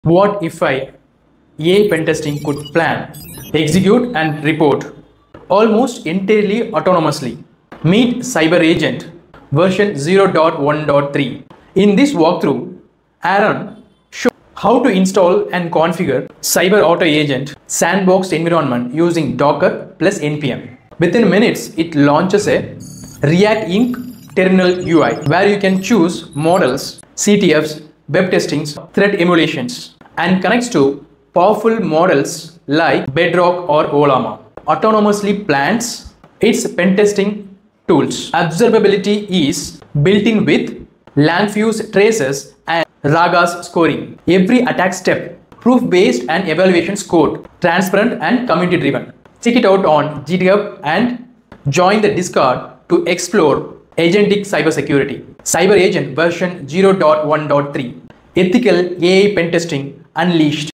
🤖𝐀𝐈 𝐏𝐞𝐧𝐭𝐞𝐬𝐭𝐢𝐧𝐠 - “𝐓𝐡𝐢𝐬 𝐀𝐈 𝐂𝐚𝐧 𝐏𝐞𝐧𝐭𝐞𝐬𝐭 𝐛𝐲 𝐈𝐭𝐬𝐞𝐥𝐟? 𝐀𝐮𝐭𝐨 𝐂𝐲𝐛𝐞𝐫𝐀𝐠𝐞𝐧𝐭 𝟎.𝟏.𝟑 𝐄𝐱𝐩𝐥𝐚𝐢𝐧𝐞𝐝!”
Imagine a cybersecurity tool that can think, explore, and test systems all on its own like a digital security agent. That’s exactly what Auto CyberAgent 0.1.3 does.